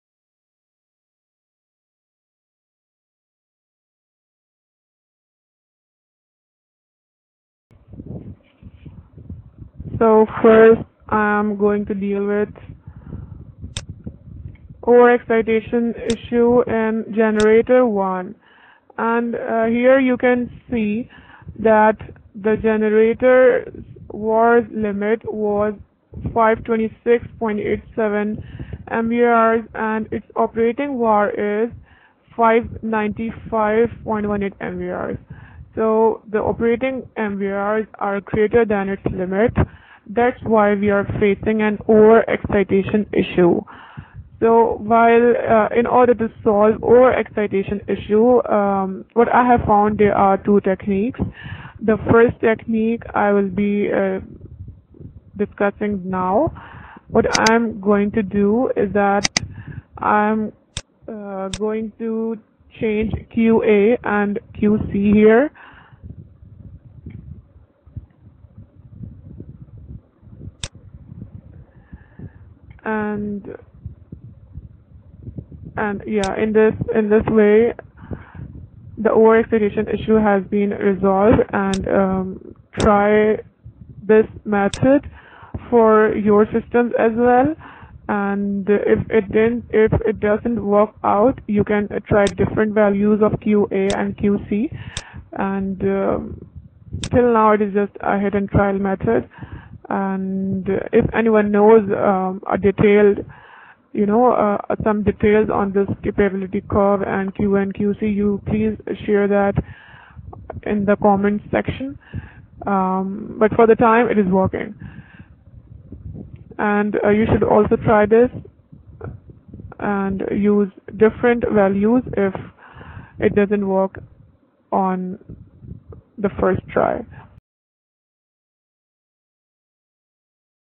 Generator Over Excitation Solution on ETAP - II
In this video, generator's over excitation error has been removed by changing  MVAR limits.